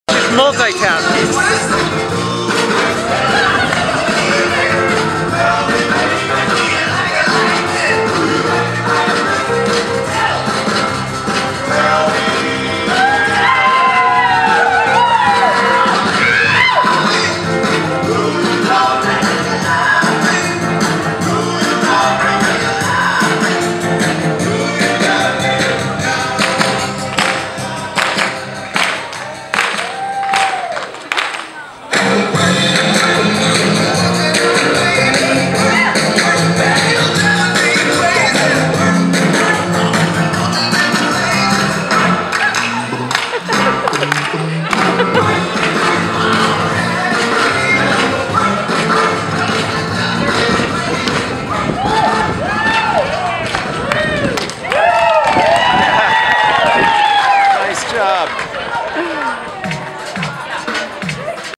Zumba Dance Party at Hot Night at the Hot Springs 2013
The Old Town Hot Springs hosted Hot Night at the Hot Springs and the night ended with a big Zumba dance party on the pool decks.